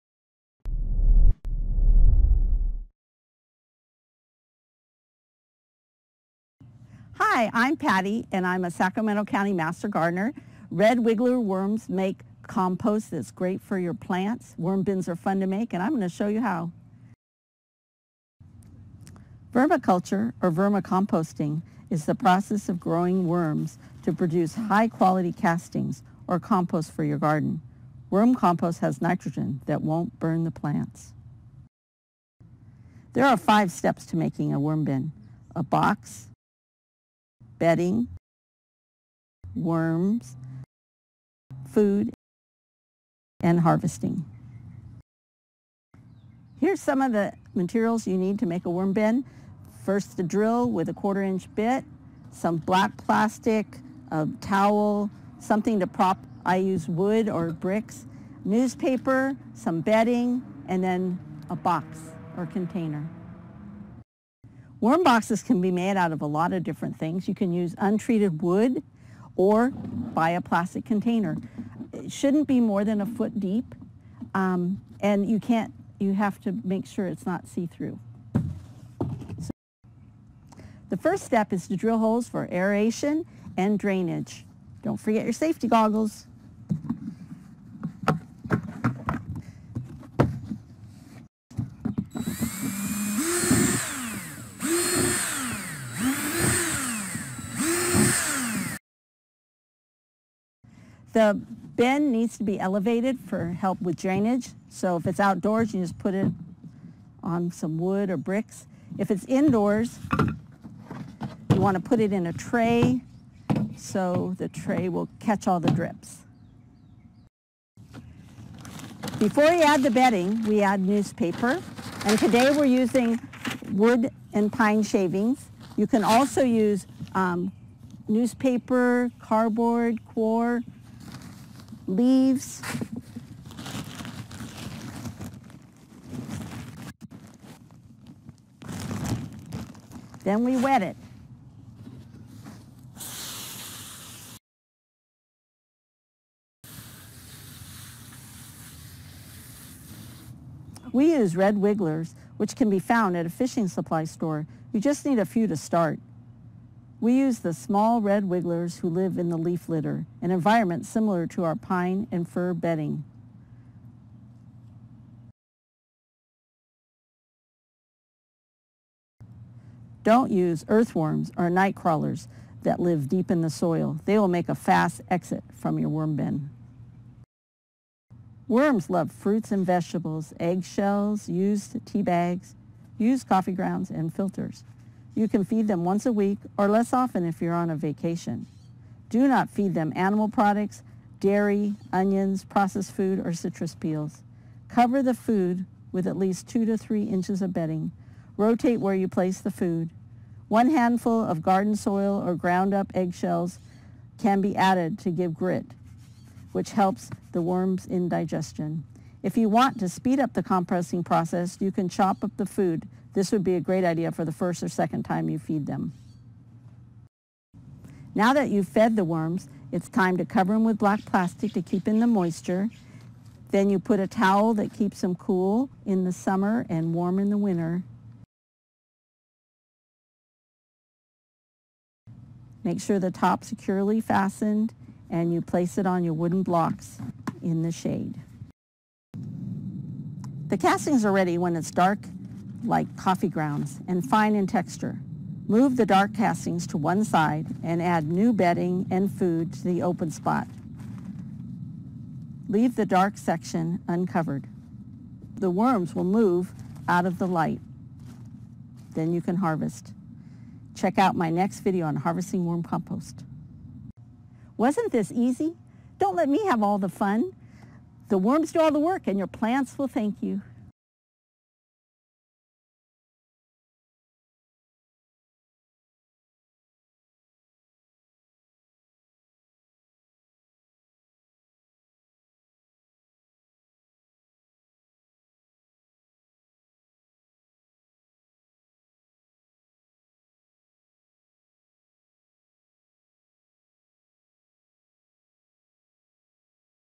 Making a Worm Bin
Learn the five steps to making a worm bin: the right container, bedding, worms, food and harvesting tips